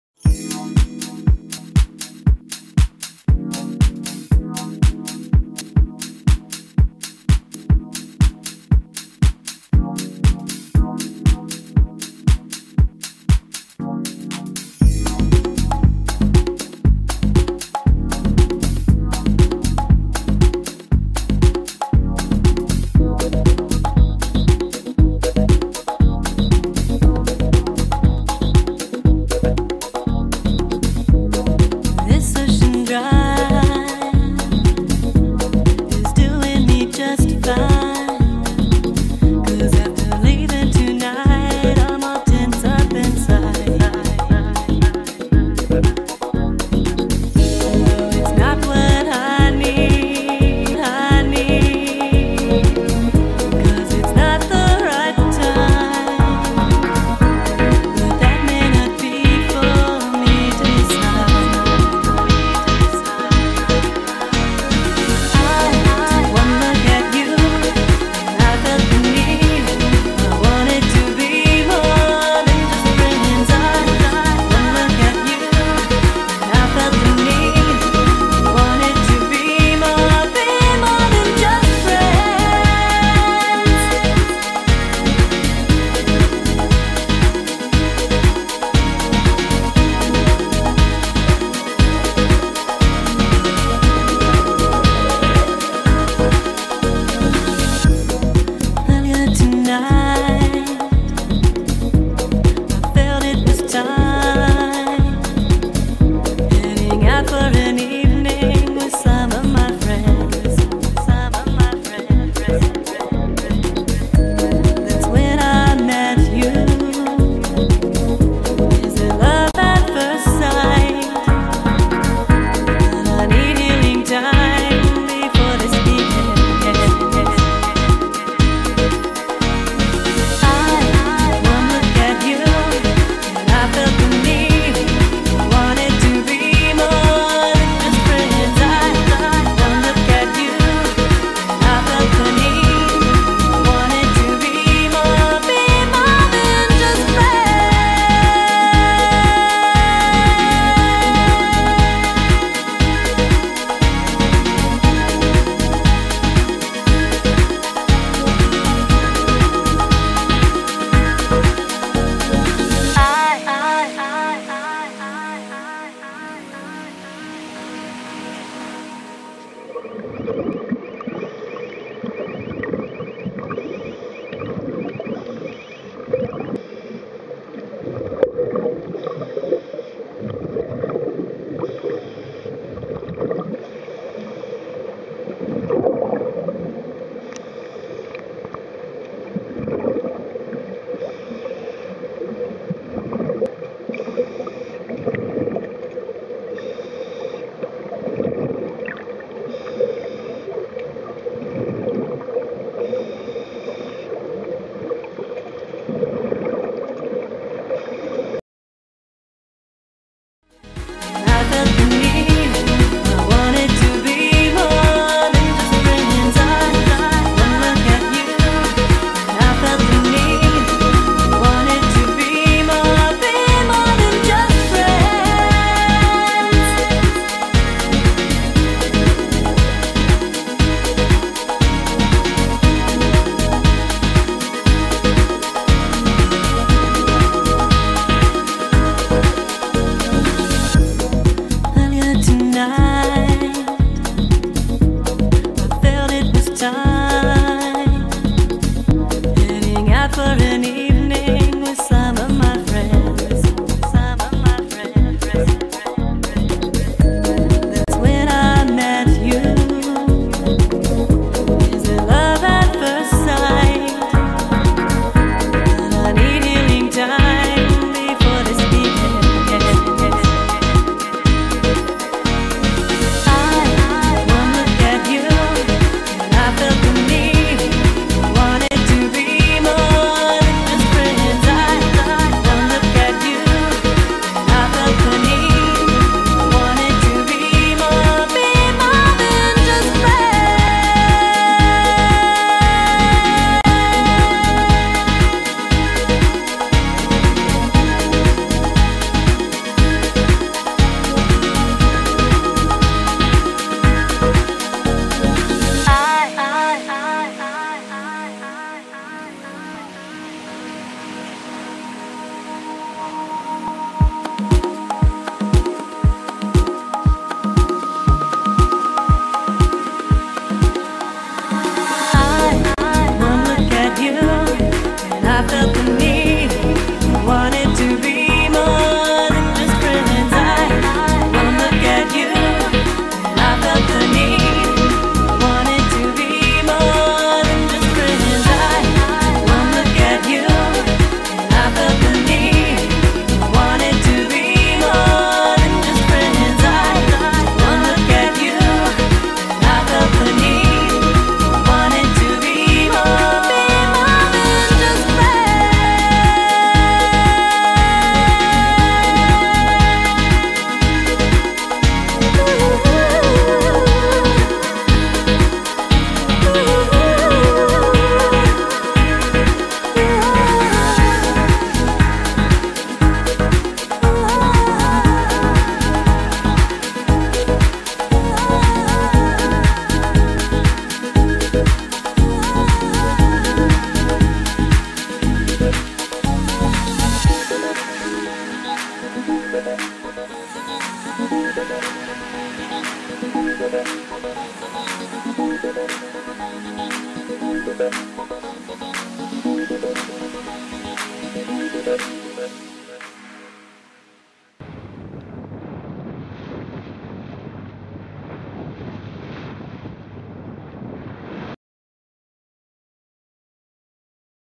Ko Losin, Thailand 泰國, 鼠島 - Scuba Diving Trip
2011/09/14 ~ 09/19 Ko Losin, Thailand
Scuba diving during 2011/9/14~19 in Koh Losin, Thailand.

Camera: Panasonic Lumix DMC-FT3
Lens: Inon UWL-H100 28LD
Light: Inon LE550-W